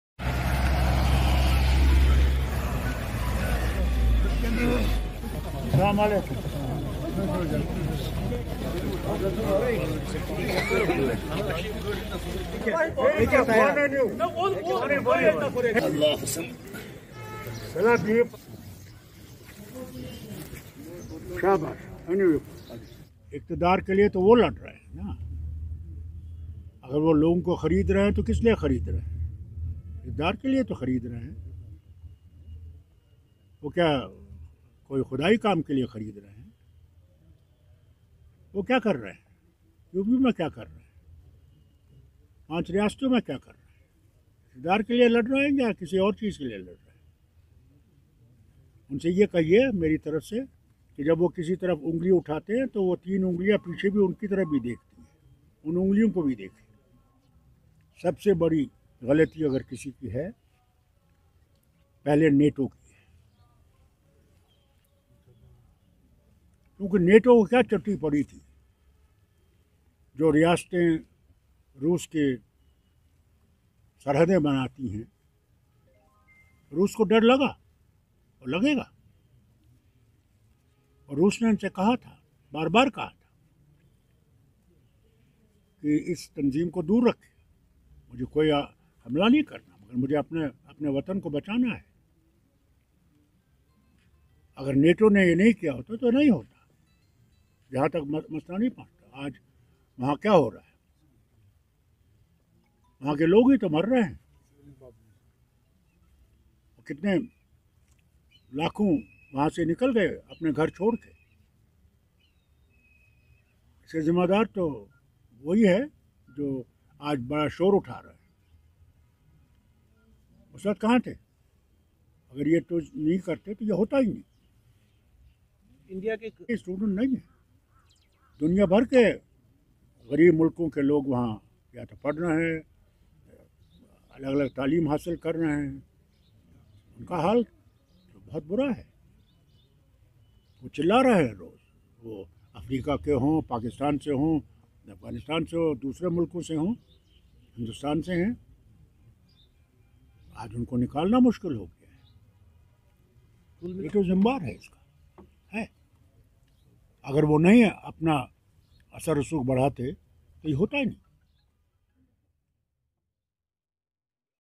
National Conference President and Former Chief Minister Dr Farooq Abdullah blamed NATO
National Conference President and Former Chief Minister Dr Farooq Abdullah blamed NATO on Ukraine sufferings. Addresses Party Functionaries at Party Headquarters Ganderbal.